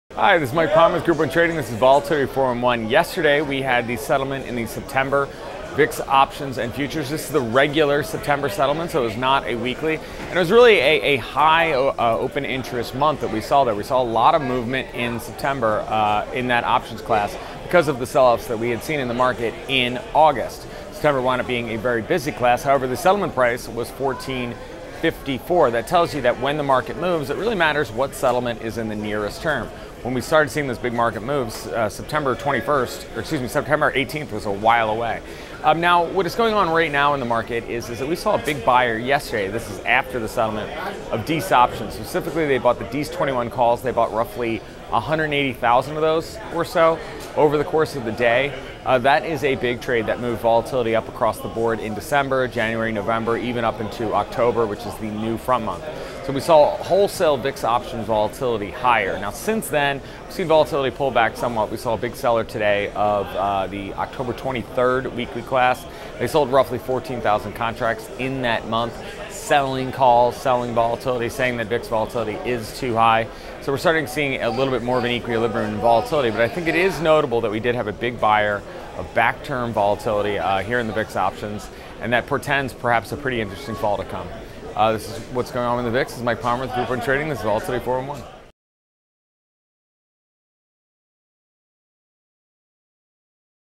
Back Term Volatility Buyer
Trader buys big in back term volatility after yesterday's $VIX options & futures settlement. Host Michael Palmer